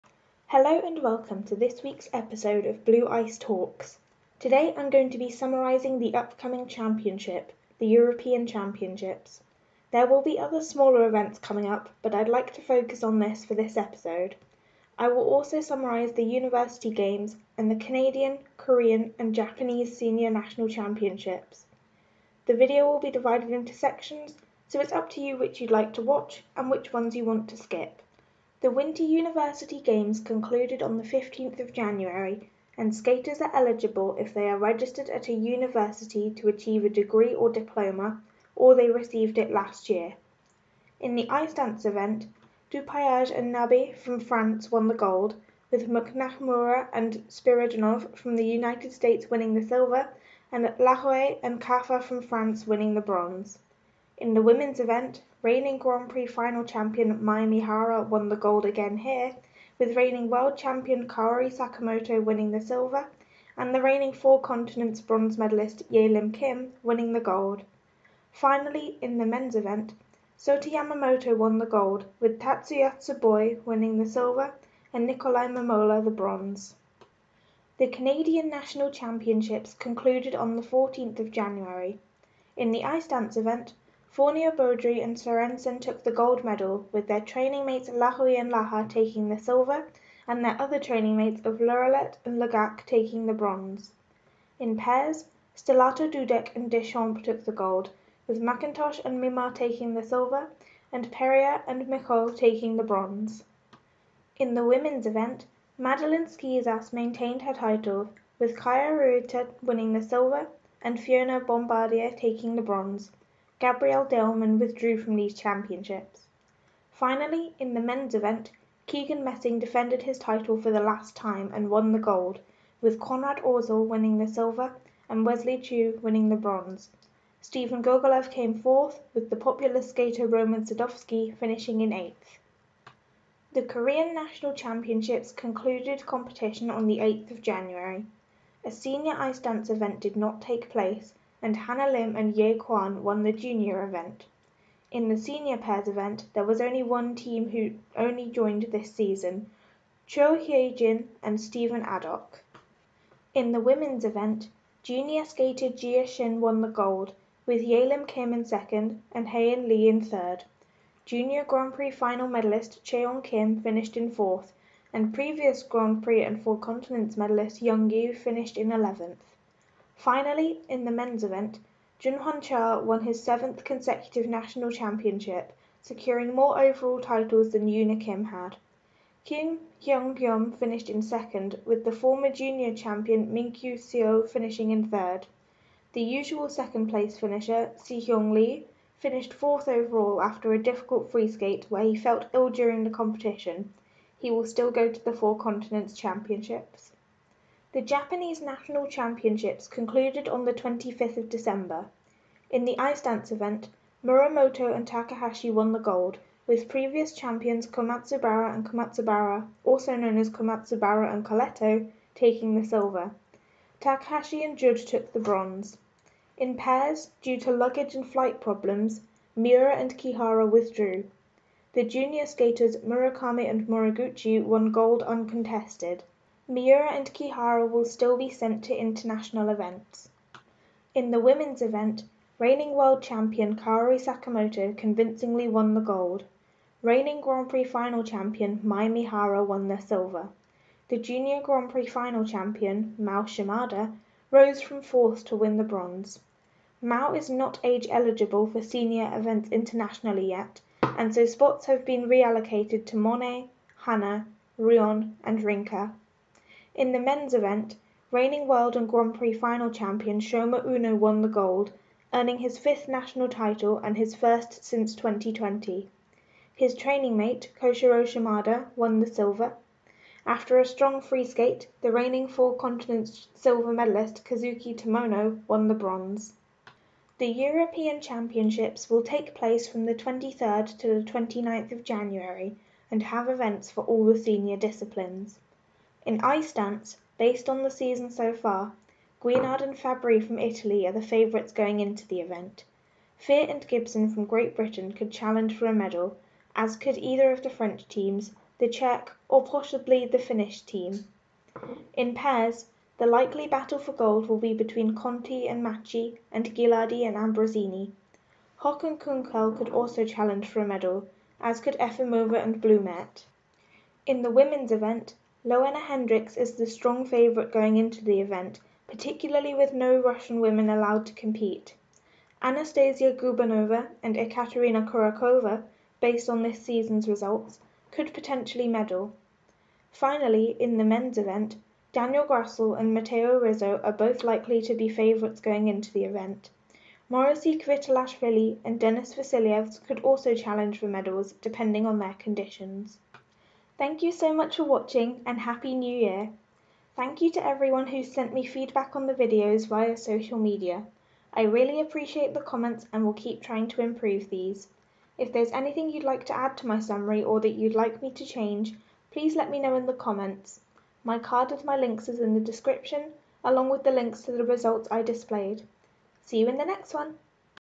Blue Ice Talks: Winter University Games, Nationals, and Europeans 2022/2023
A summary of the Winter University Games, and Canadian, Korean, and Japanese skating national championships. I am sorry if I mispronounce any names!

Timestamps:
Intro: 00:00
University games: 00:28
Canadian Nationals: 01:17
Korean Nationals: 02:13
Japanese Nationals: 03:17
European Championships: 04:37
Endscreen: 05:43

Korean national results: For these, I would suggest looking at skatingkor on twitter